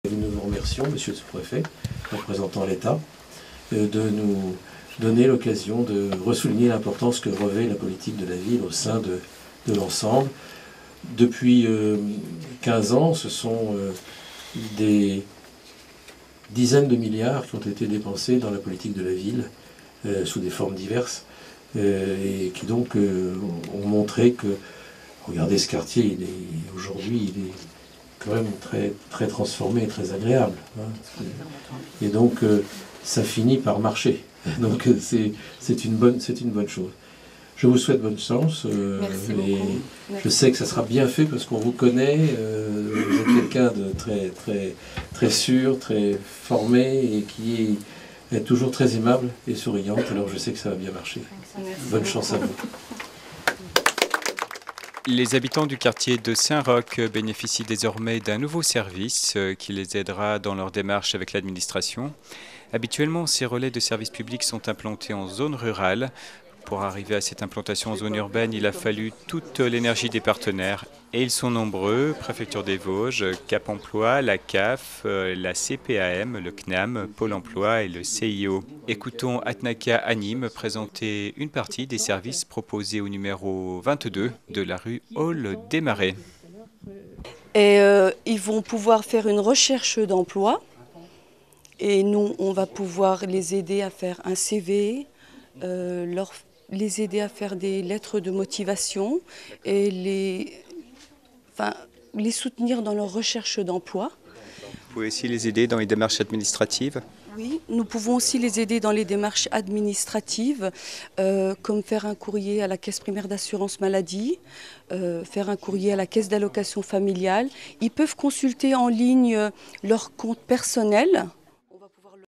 Relais de services publics
Les habitants du quartier de Saint Roch bénéficient désormais d'un nouveau service qui les aidera dans leurs démarches avec l'administration
Habituellement ces Relais de services publics sont implantés en zone rurale
pour arriver à cette implantation en zone urbaine, Il a fallu toute l'énergie des partenaires.
ils sont nombreux, préfecture des Vosges, Cap Emploi, la CAF, la CPAM, la CNAM, Pole Emploi, le CIO.
Ecoutons Atnaca Hanim, présenter une partie des services proposés au n 22 de la rue Olh des Marais